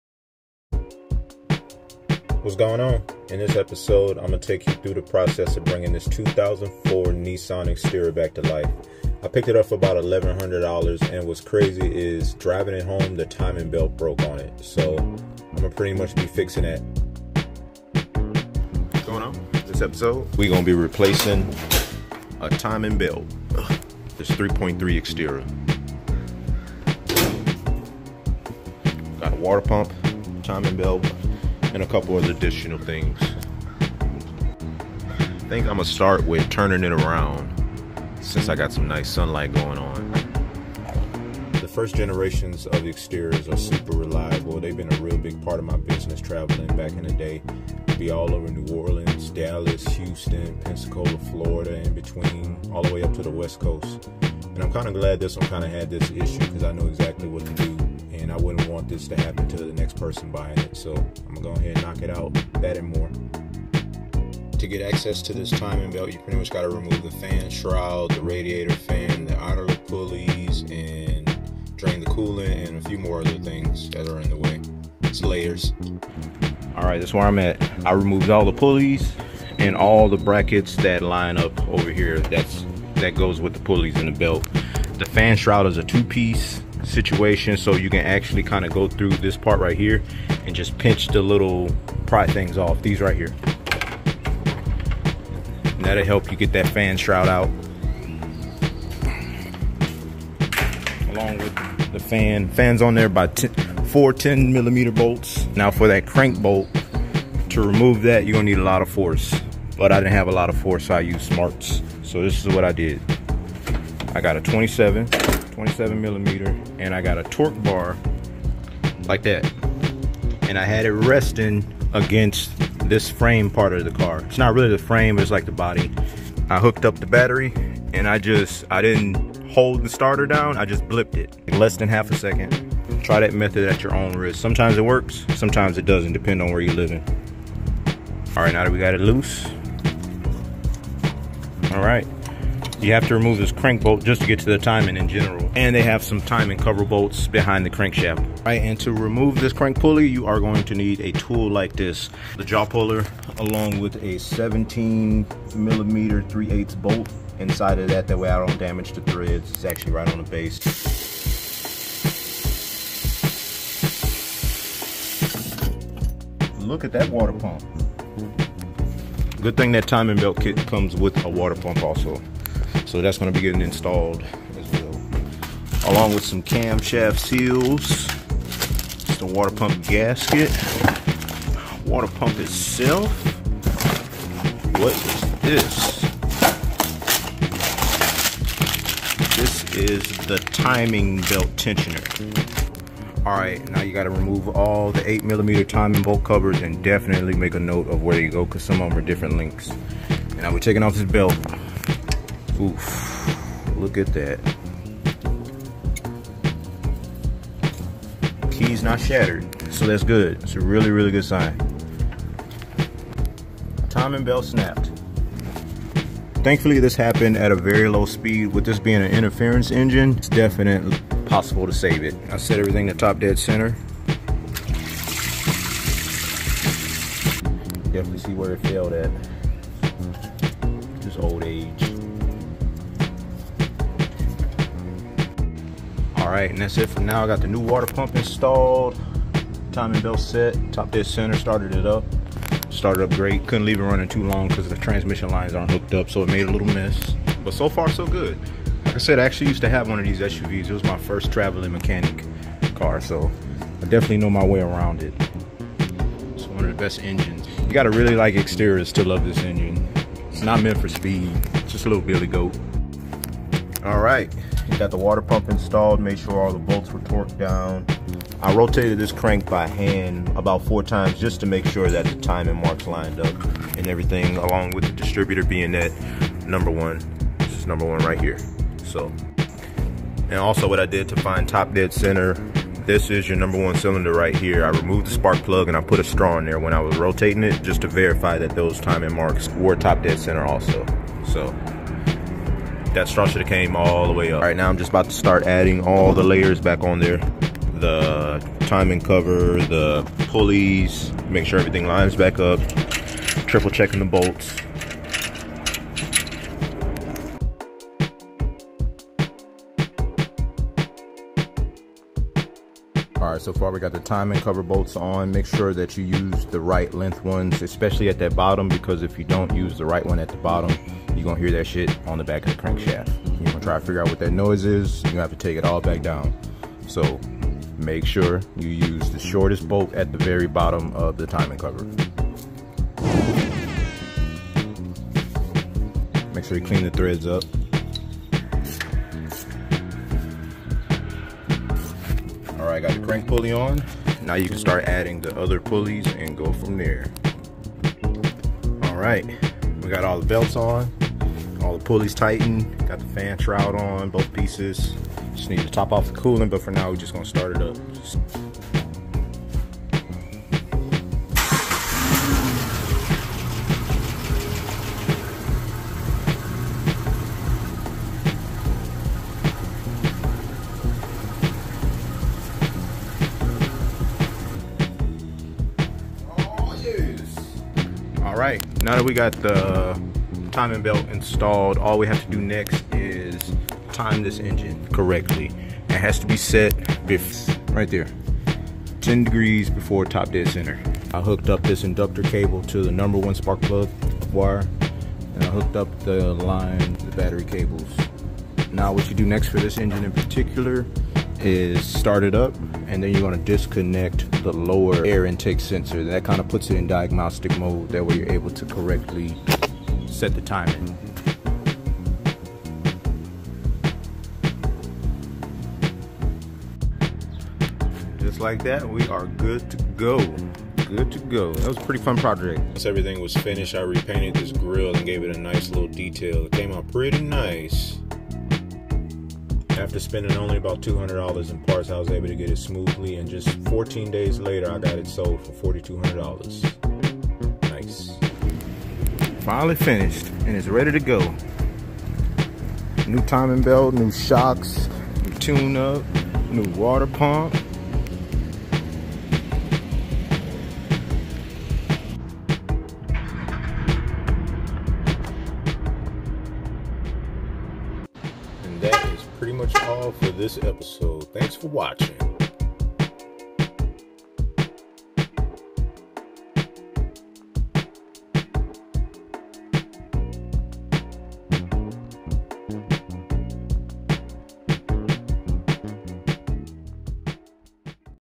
Xterra Transformation: Turning a $1100 Beater into $4200 Gem | HerbzAutoCare
In this episode of Herbz Auto Care, join me as I take you through the restoration journey of a 2004 Nissan Xterra. I picked up this first-gen Xterra for just $1100, thinking it would be a straightforward project.
However, things took an unexpected turn when the timing belt snapped on the drive home.

First-gen Xterras hold a special place in my heart. They've been reliable workhorses for my business travels across Dallas, New Orleans, and Houston.

This  Xterra needed some serious attention.
Throughout the video, you'll see the detailed process of replacing the timing belt, water pump, and distributor to bring this SUV back to life.

 It's not just about making a quick flip; it's about ensuring every vehicle I work on is safe and reliable for the next owner.

After investing another $200 in parts and putting in the necessary effort, I got the Xterra running smoothly again. Two weeks later, it found a new owner who appreciated the quality work that went into its restoration.

Watch as I navigate the challenges and triumphs of this restoration project. Whether you're a car enthusiast, a DIY mechanic, or someone who loves a good comeback story, this episode has something for you.